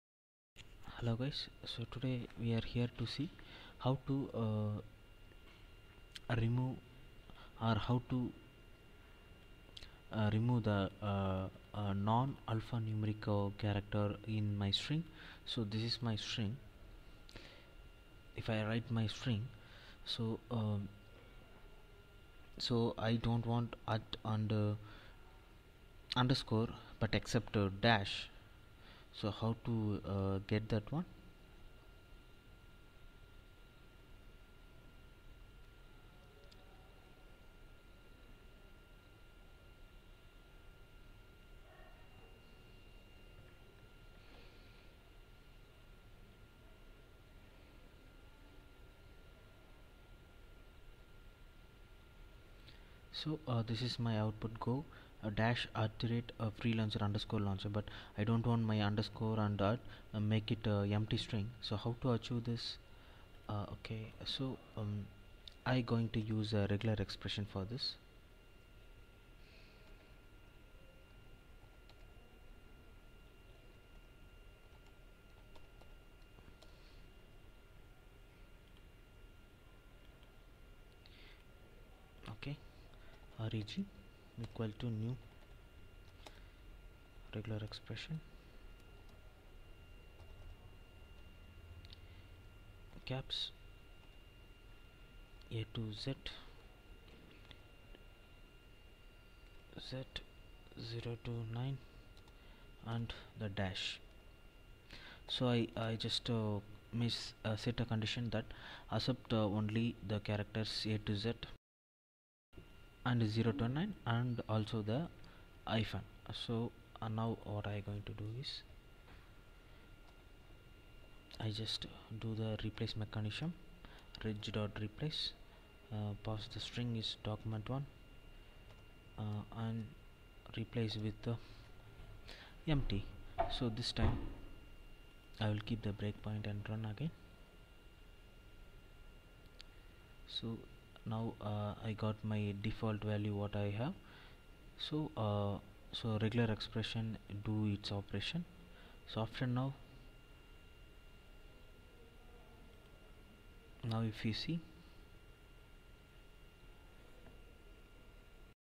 How do I remove all non alphanumeric characters from a string except dash
Replace [^a-zA-Z0-9 -] with an empty string.
May be you like this